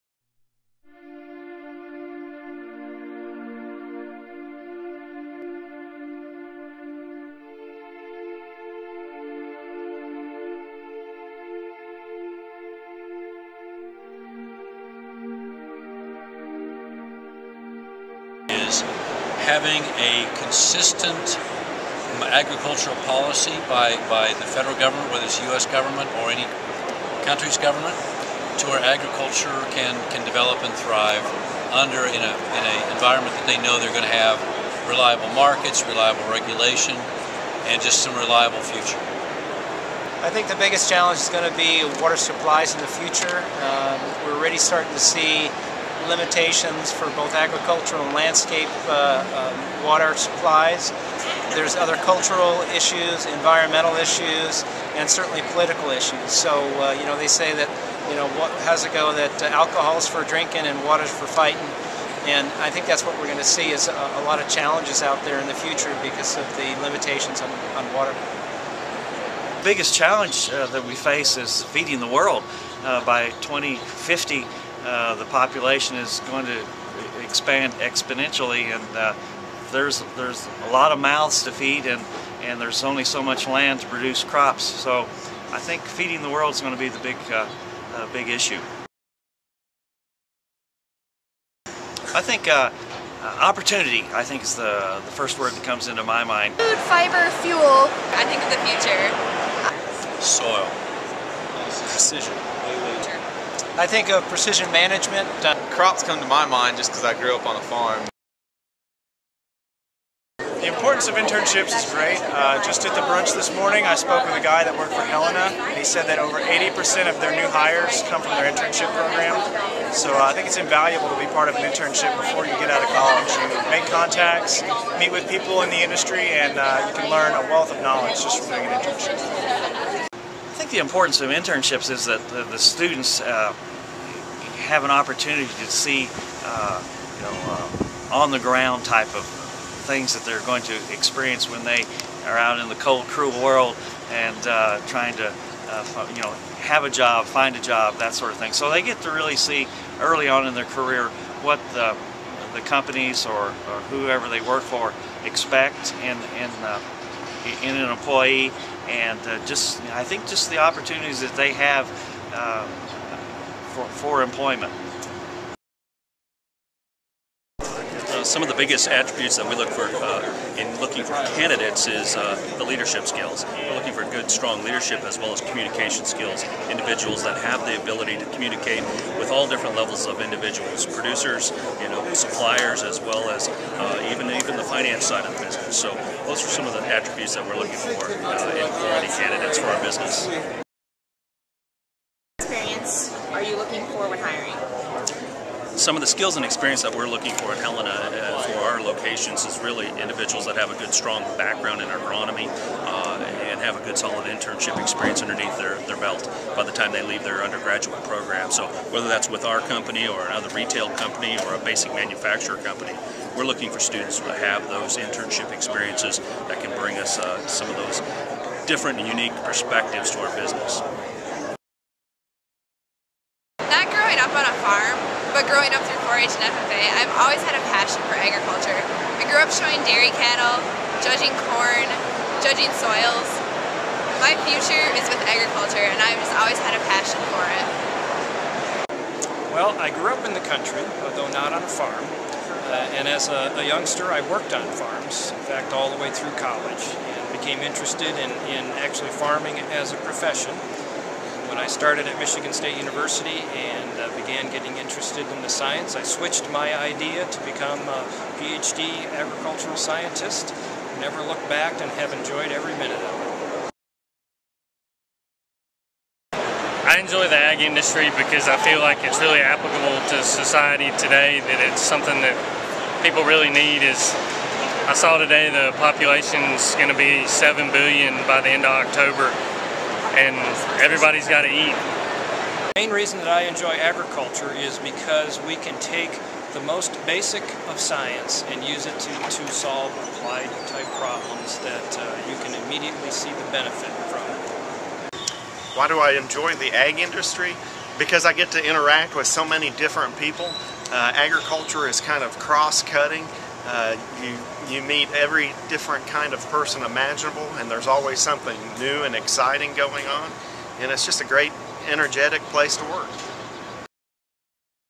2011 SASES Video Challenge Kansas State
During the 2011 Agronomy Meetings students from Kansas State University interviewed professionals about careers in agronomy.